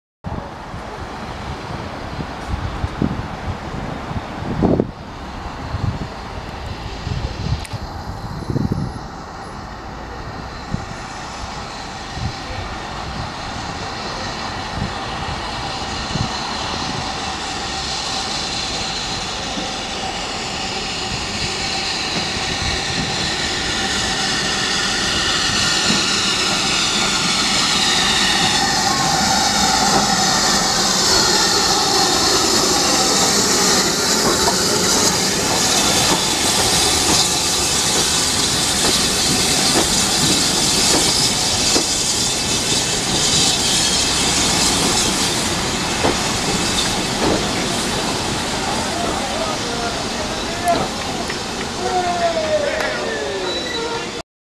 80097 on the East Lancs Railway
Std 4MT 80097 arriving at Ramsbottom on the East Lancs railway.

No there are no masks in sight, as this was taken last year, and haven't been able to make it this year.